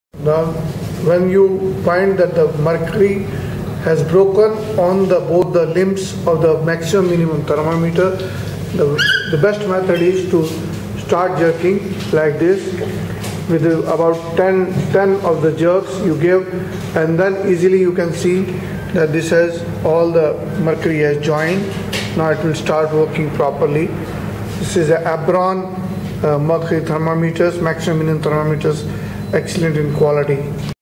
Video Maximum minimum thermometer repair broken mercury info@abronexports com
how to repair or set broken mercury in maximum and minimum thermometer Demo Video by Abron exports for detail write to